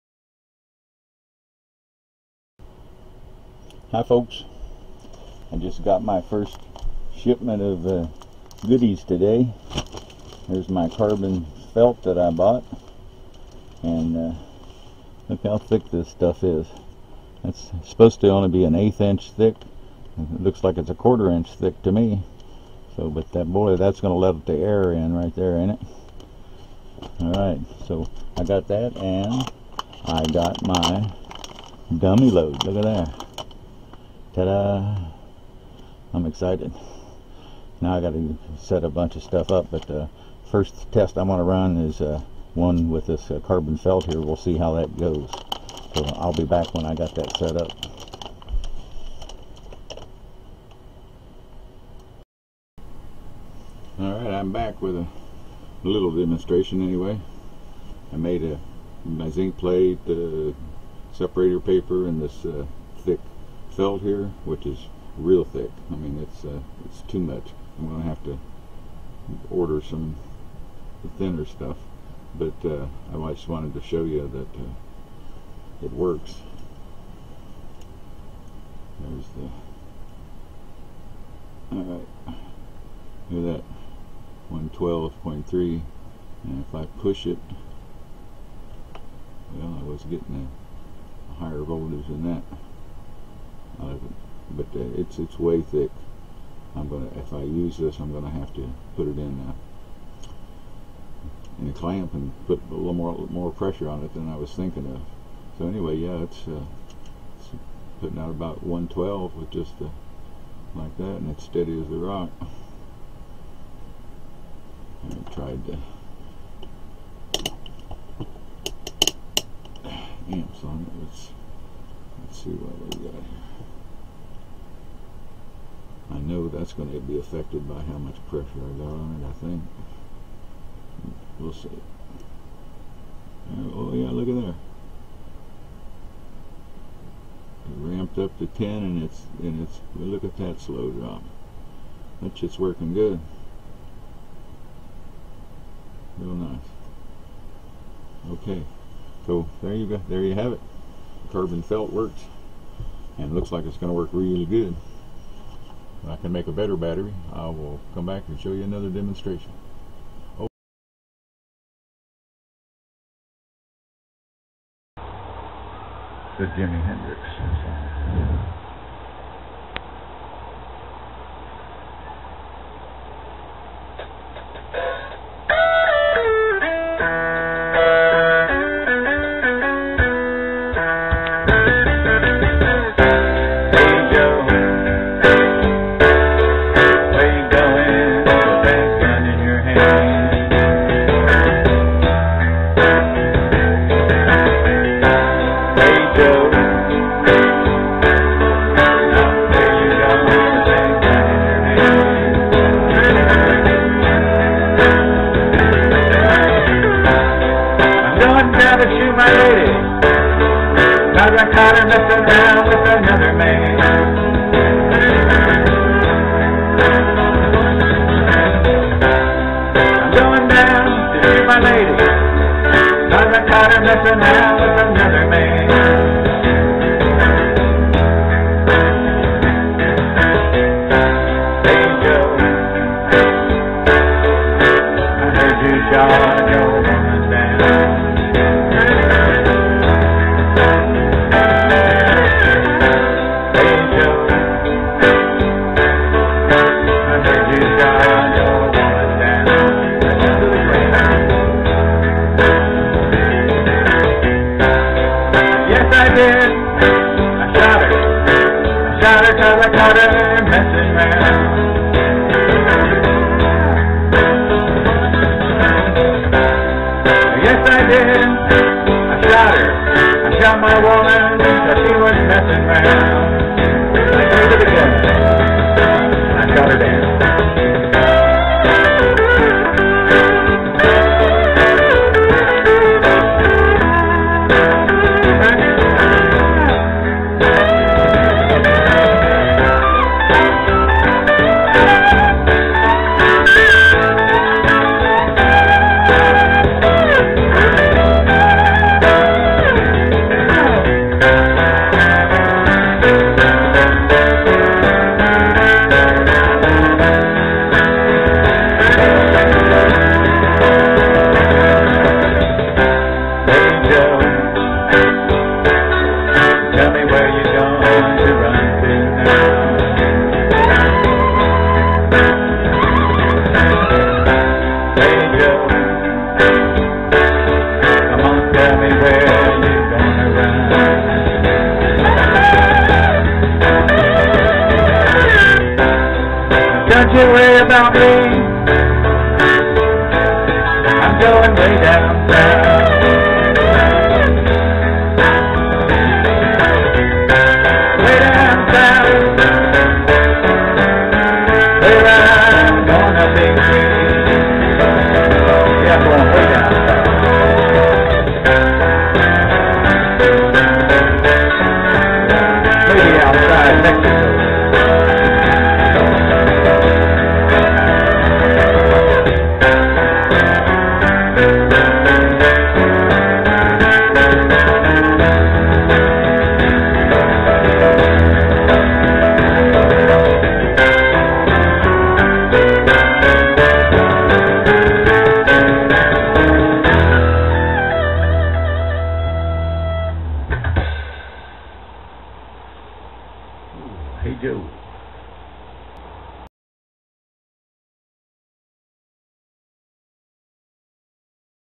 New Carbon Felt Cathode
Testing the new carbon felt cathode.